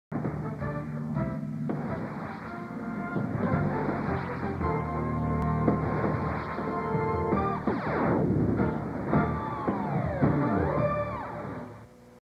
VINHETA - REDE MULHER - 2002
Vinheta do antigo canal Rede Mulher, hoje Record News, provavelmente em 2002.